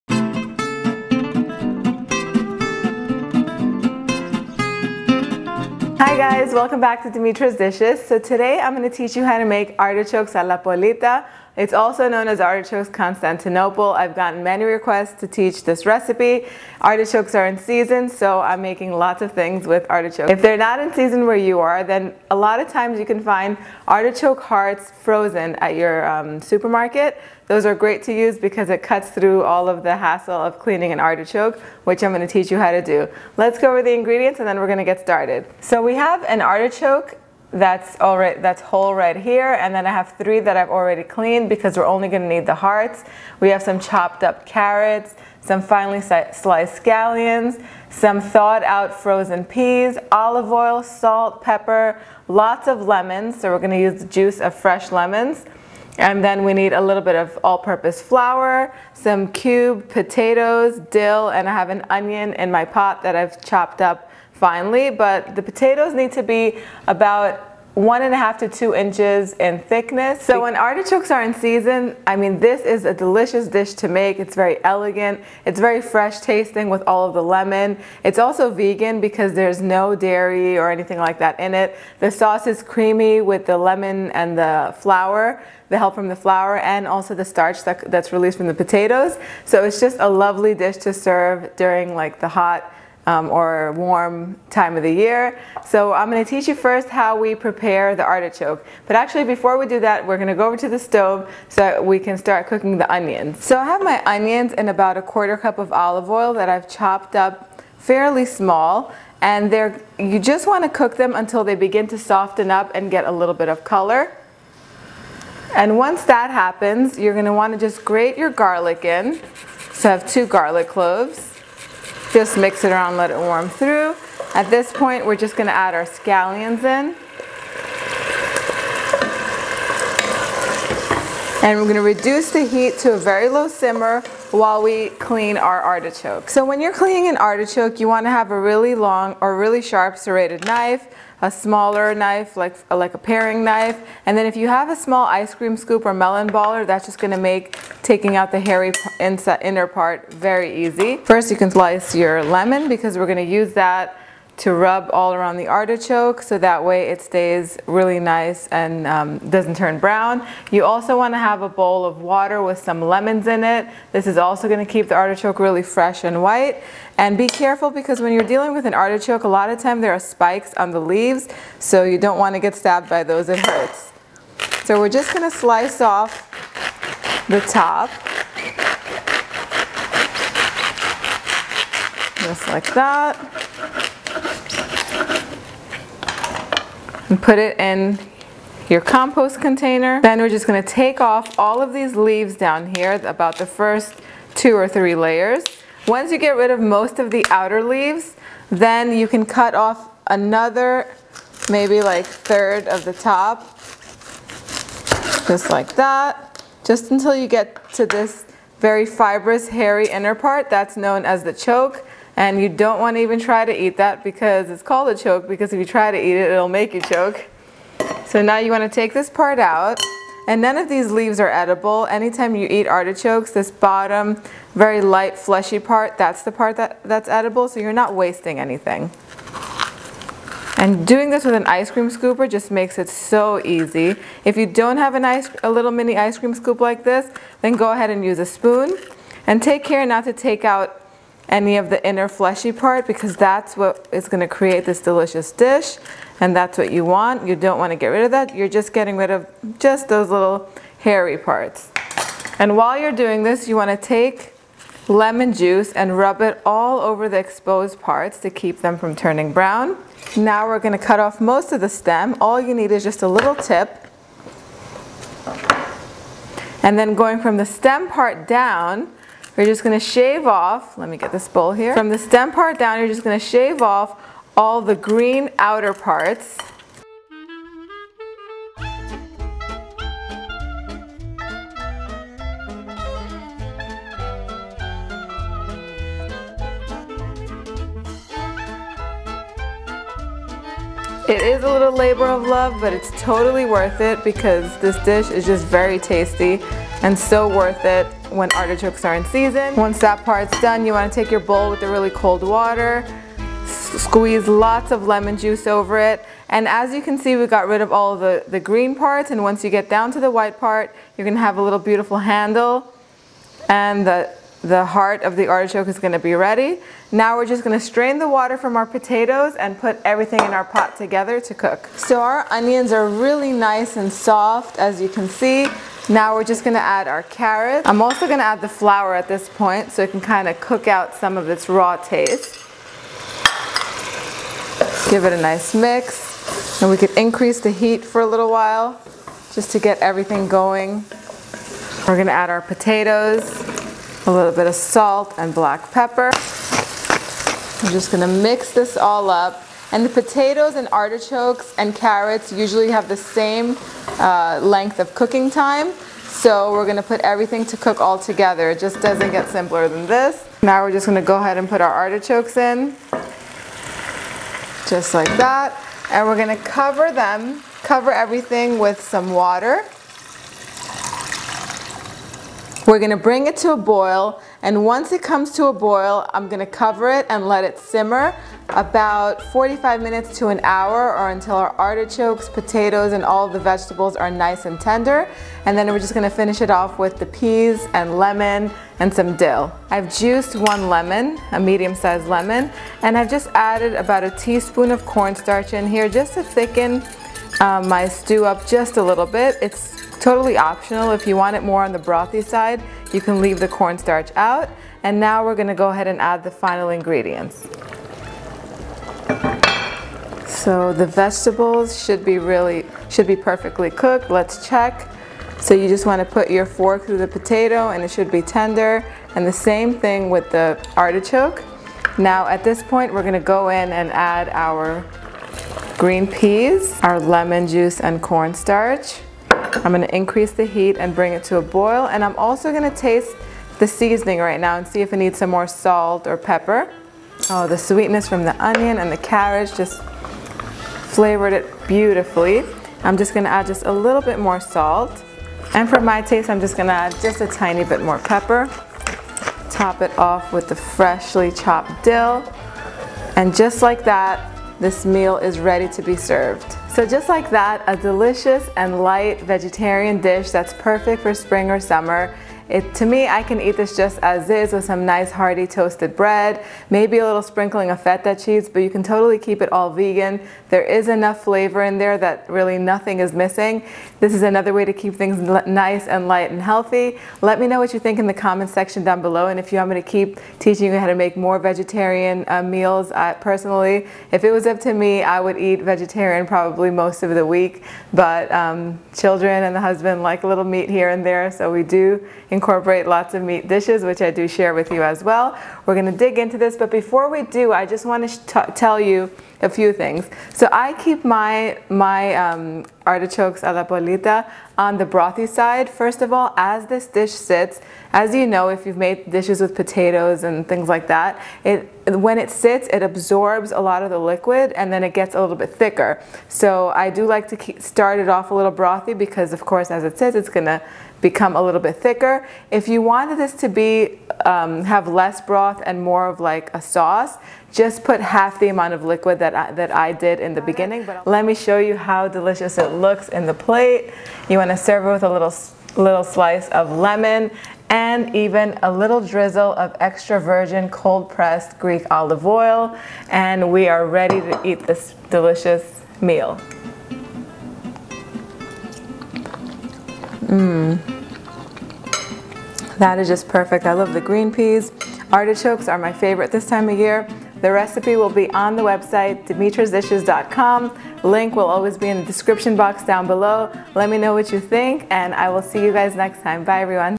Artichokes a la Polita: Greek Style Artichoke (Constantinople) Stew
Serves 4-6:
4-6 fresh artichokes
½ - 1/3 cup olive oil
1 small onion, finely chopped
2 garlic cloves, grated
8 scallions, thinly sliced
2 carrots, sliced
4 small – medium potatoes, quartered
1 tablespoon all-purpose flour
1 cup frozen green peas, thawed
¼ cup finely chopped fresh dill
Salt and freshly ground pepper, to taste
1/3 cup fresh lemon juice (or less)
1-2 lemons, sliced in half for rubbing on artichokes
1 teaspoon cornstarch (optional)

Cook the finely chopped onion with the olive oil in a pot over low heat until soft and golden. About 10-15 minutes.

In the meantime, carefully peel the artichokes.

Keep a large bowl halfway filled with ice water and 1-2 lemons cut in half inside the water.

Remove the outer leaves of the artichoke and the hairy choke. Rub it all over with lemon and place in the bowl of water. This will keep them from turning brown.

Add the grated garlic to the pot and warm through. Add scallions and cook over low heat until soft. About 5 minutes.

Add the carrot and all-purpose flour. Mix it and cook for 1-2 minutes over high heat.

Add the artichokes, potatoes and season with salt and pepper.
Pour enough water in the pot to almost cover the vegetables.
Bring to a boil then cover the pot with the lid. Reduce to a medium low heat and cook about 45 minutes or until the vegetables are fully cooked and tender.
Taste and add more salt and pepper in needed.
Add the green peas and stir to combine.

Combine the lemon juice with the cornstarch in a small bowl and mix to dissolve. Begin with a small quantity of lemon juice (1/4 cup) because sometimes lemons are very strong and somewhat bitter. You can always add more lemon juice afterwards.

Note: If you prefer your artichoke stew more on the brothy side, do not add the cornstarch. The addition of cornstarch gives it a creamy velvety texture while thickening it a little bit.

Add the lemon juice and cornstarch mixture to the pot and mix to combine.

Bring to a boil and taste once again for seasoning and add more lemon if desired.

Top with the chopped fresh dill and serve.

The stew will thicken as it sits and tastes wonderful with toasted bread. Enjoy!